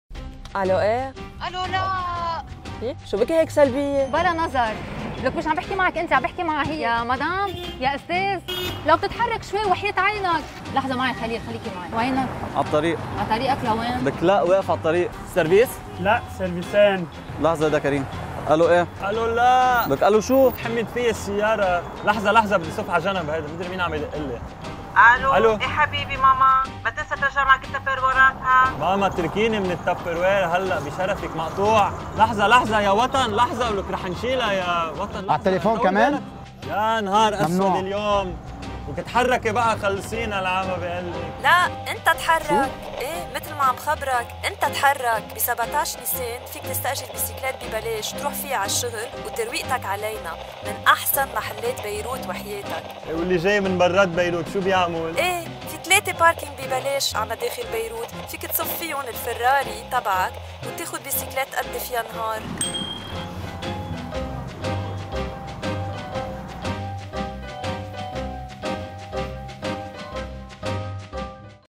Think Outside The Box 🚗
Time to BEAT THE TRAFFIC!

Script by Michelle Keserwany
Video Shot & Edited by Création Neuf
Special Thanks To Lebanese internal security forces & to all volunteers with whom we enjoyed making this video

Bike To Work 2019 is under the patronage of وزارة البيئة - لبنان and with the support of Embassy of Switzerland in Lebanon & the Lebanese American University - LAU